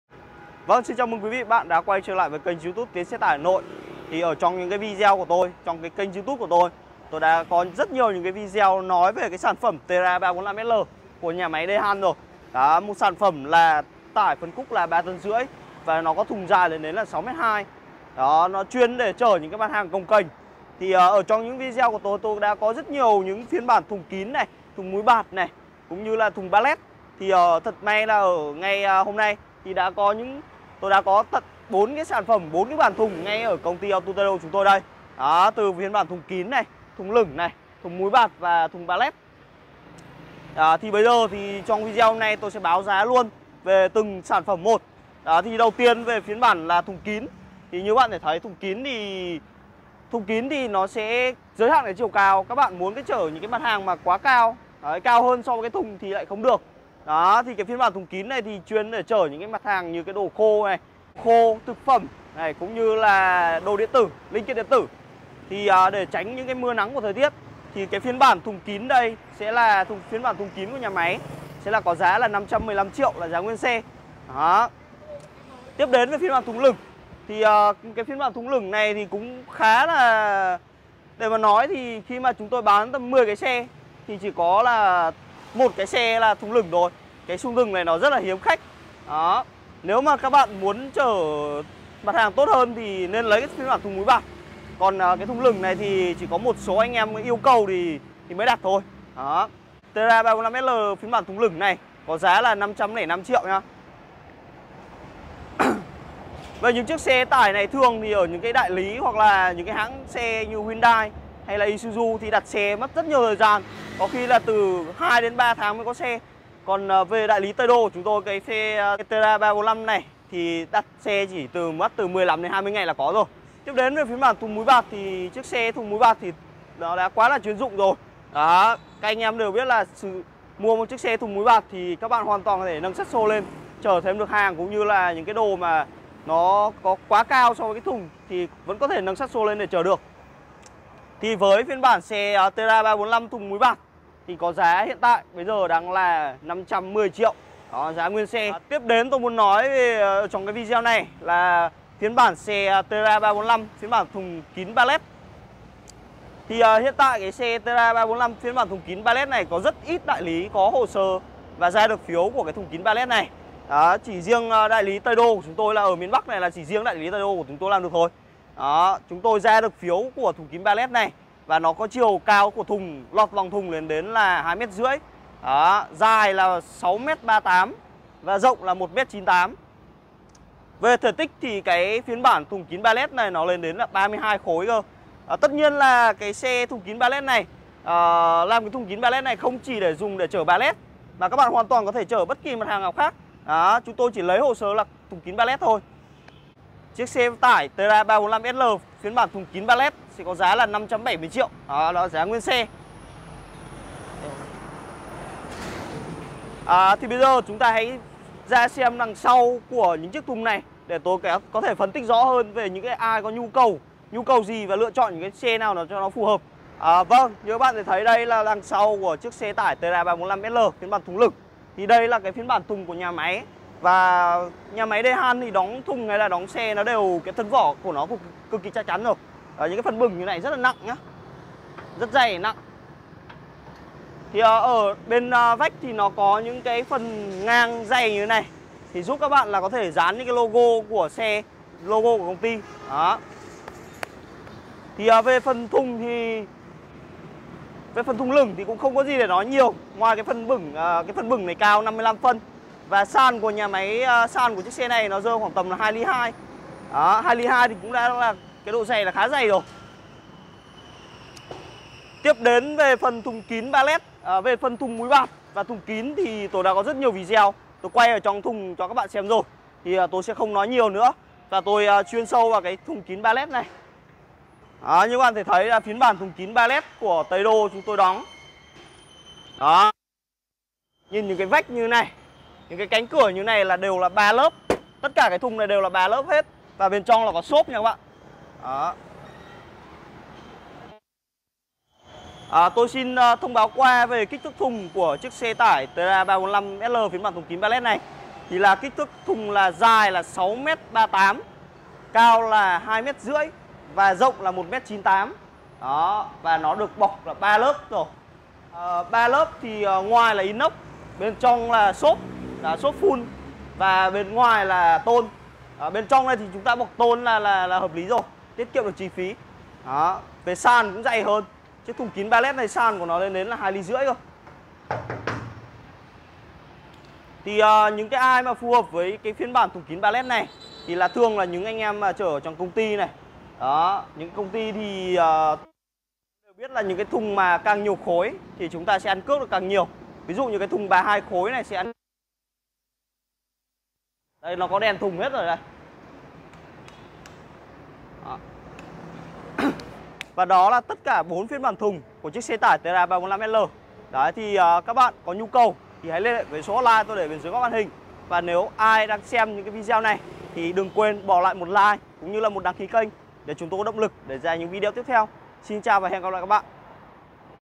Báo giá xe tải Tera 345sl 3,5 tấn thùng dài 6m2
Xe tải Tera 350 là phiên bản khác của sản phẩm Tera 345sl của tập đoàn Daehan motor
Xe sử dụng động cơ ISUZU J4 .
Với công suất cực đại là 110 ps
Dung tích xi lanh: 2.770 cc
Momen xoắn : 270 Nm
Một động cơ tốt đã được rất nhiều nhà máy lắp ráp như : Jac , Đô thành , Teraco
Xe với thùng dài 5m
Phù hợp với tất cả mọi mặt hàng trên thị trường
Địa chỉ: 883 Quảng Trung Hà Đông Hà Nội